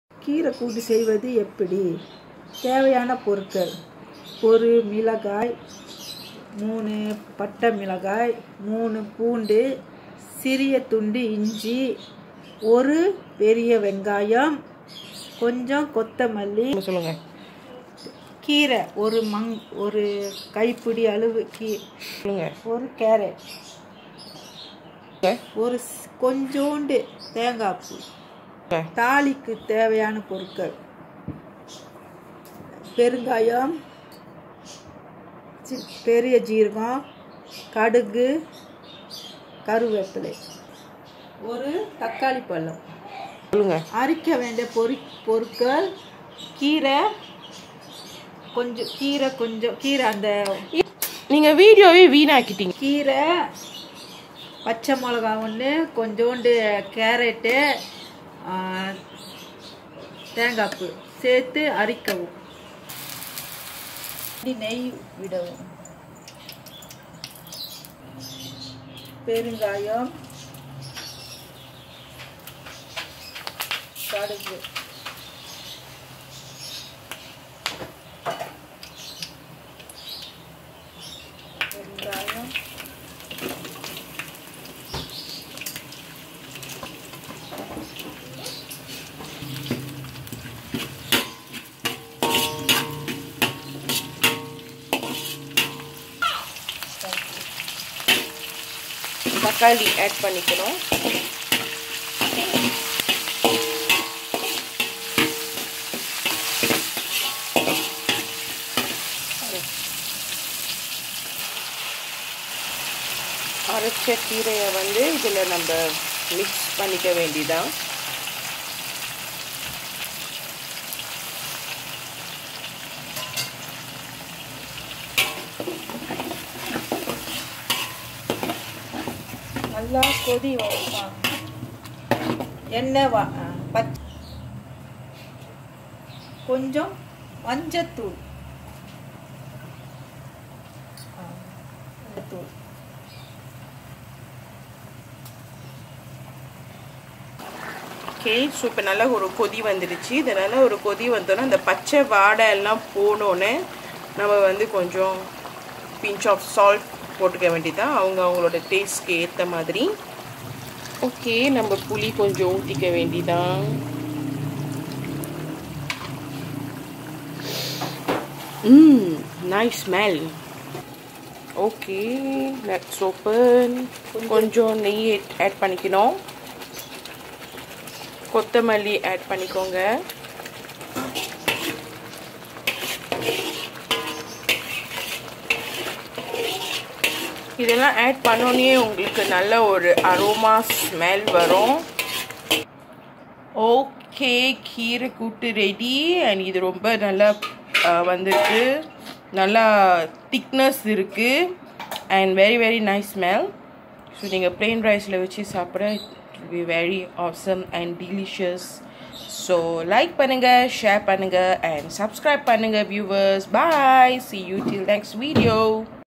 Kirai koothe ( Spinach koothe ). Steps in description 👍👌
Main Ingredient :
**Bayam ( Spinach )
**1 Carrot
**handful of Grated Coconut
**1 Green / Red chilli ( according to spice taste)

Saute Ingredients :
**Ginger
**Garlic
**Dry Chillies
**1 Onion
**1 Tomato 🍅
**1 Table Spoon Ghee
**Tamarind water
**Asafoetida ( Perungayam )
**Mustard Seeds ( biji sawi )
**Cumin ( Jintan )
**Curry Leaves

Add on taste : Pinch of Salt

For add on Aroma : Coriander Leave ( Daun Ketumbar )

Cooking Methods :

Step 1:
Saute till golden brown all those saute ingredients except Asafoetida and Tamarind.

Step 2:
Blend those main ingredients by adding a small cup of water

Step 3:
Mix the blended Spinach koothe with the saute ingredients and add on salt with Asafoetida and Tamarind and let it boil till it cooked.

Step 4:
Tadaaaaaaa... Its simple and easy and ready to serve.